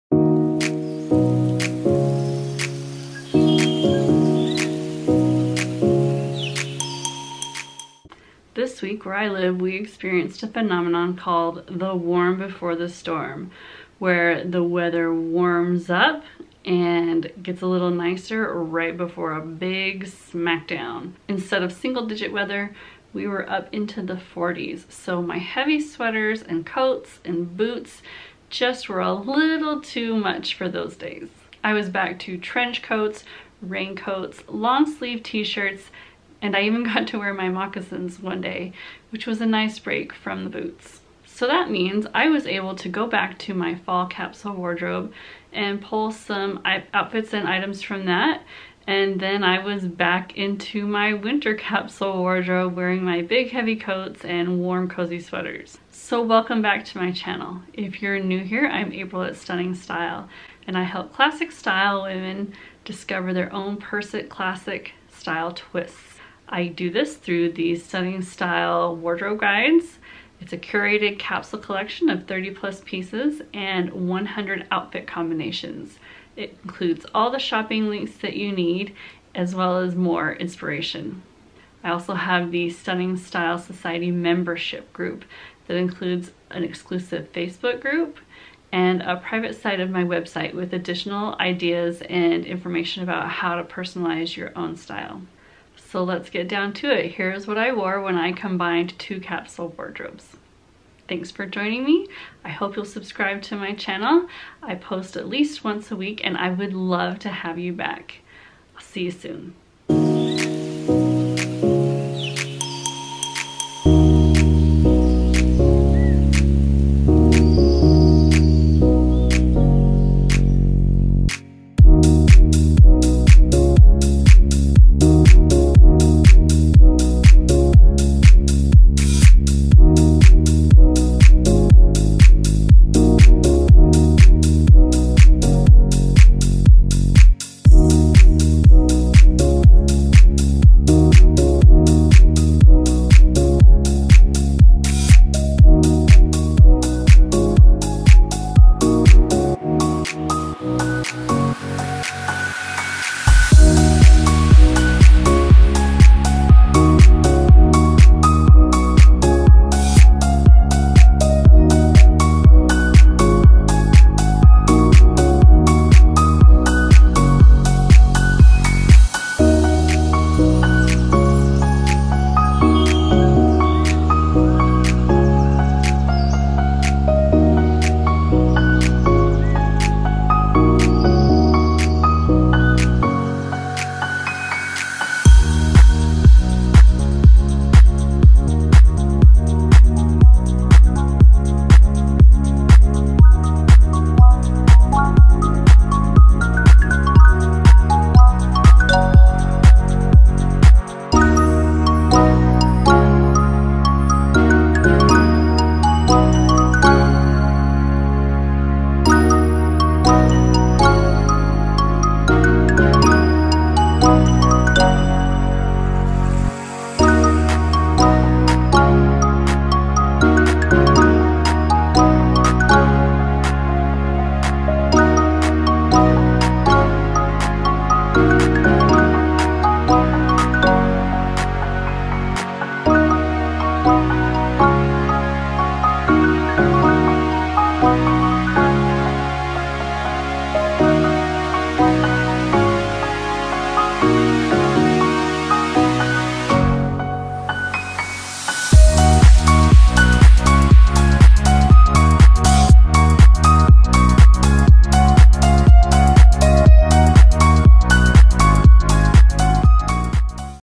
What I Wore When I Combined 2 Capsule Wardrobes
This week we had some cold days, and a warm before the storm, so it was more like fall weather. I got to pull out my trench coat and a rain jacket. I even went sans coat a few days.

But then the storm came, the temps dropped and I’m bundled up just like I was before. I enjoyed the little break, though. It was so nice to dive back into my fall capsule wardrobe for a few days, and even mix the pieces from both wardrobes.